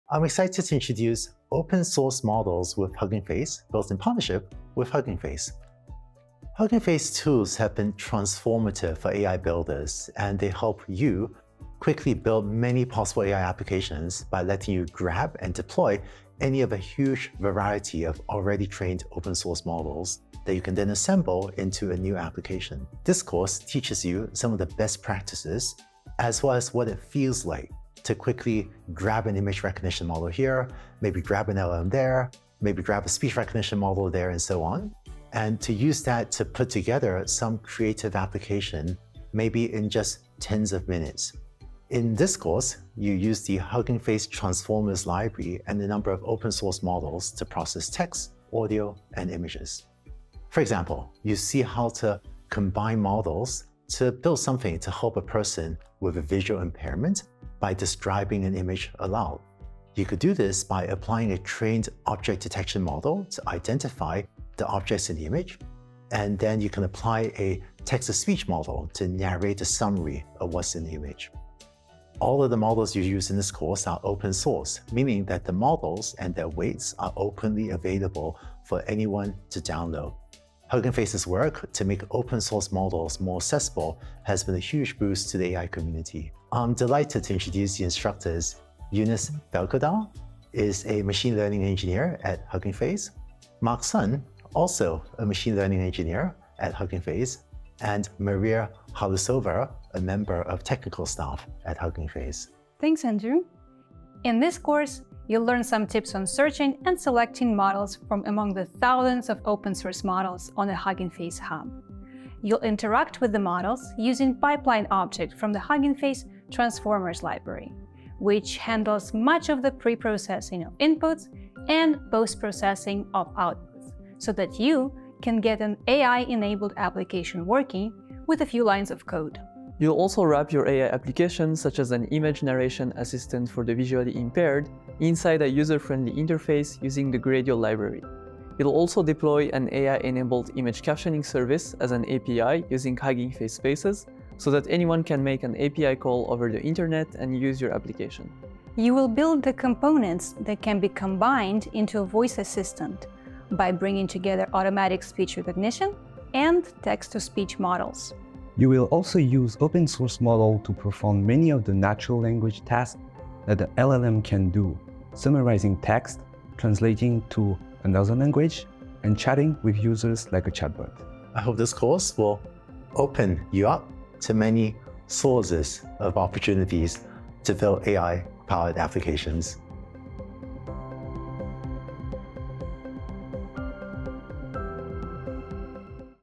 New course with Hugging Face: Open Source Models with Hugging Face
Introducing a new short course made in collaboration with @HuggingFace: Open Source Models with Hugging Face.

In this course, you’ll learn how to find and filter open source models on Hugging Face Hub to perform text, audio, image, and multimodal tasks using the Hugging Face transformers library. You’ll also learn to easily share your AI apps with a user-friendly interface or via API and run them on the cloud using Gradio and Hugging Face Spaces.

What you’ll do in this course:

- Use the transformers library to turn a small language model into a chatbot capable of multi-turn conversations to answer follow-up questions.
- Translate between languages, summarize documents, and measure the similarity between two pieces of text, which can be used for search and retrieval.
- Convert audio to text with Automatic Speech Recognition (ASR), and convert text to audio using Text to Speech (TTS).
- Perform zero-shot audio classification, to classify audio without fine-tuning the model.
- Generate an audio narration describing an image by combining object detection and text-to-speech models.
- Identify objects or regions in an image by prompting a zero-shot image segmentation model with points to identify the object that you want to select.

The course will provide you with the building blocks that you can combine into a pipeline to build your AI-enabled applications!